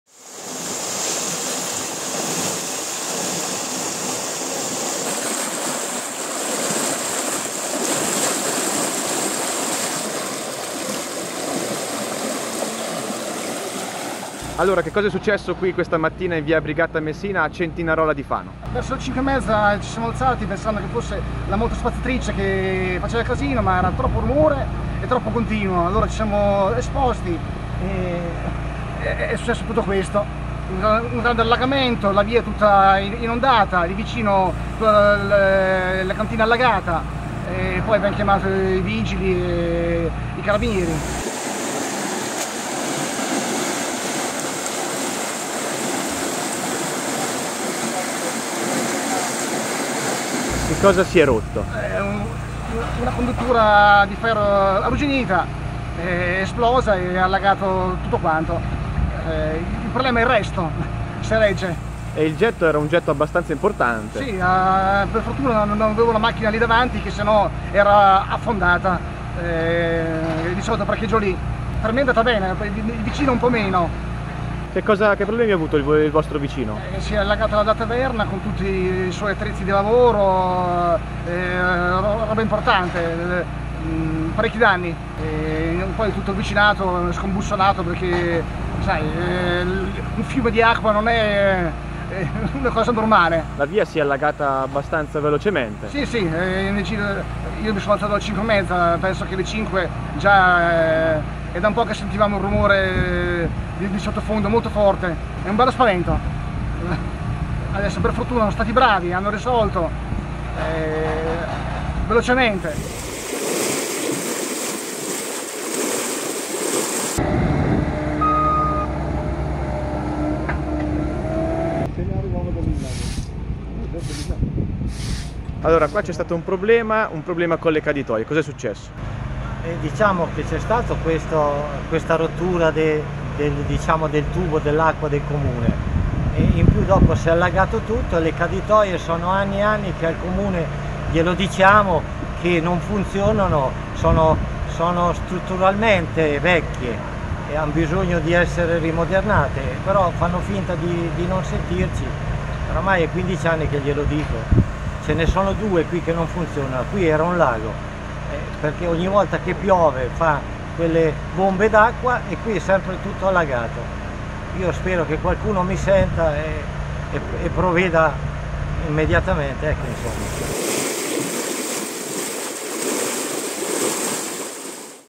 Rottura tubatura idrica a Centinarola di Fano: strada e scantinati allagati
Importante rottura di una tubazione dell’acquedotto cittadino nella prima mattinata di oggi in via Brigata Messina a Centinarola di Fano. La strada è stata chiusa al traffico per permettere la sistemazione del guasto da parte dei tecnici intervenuti sul posto.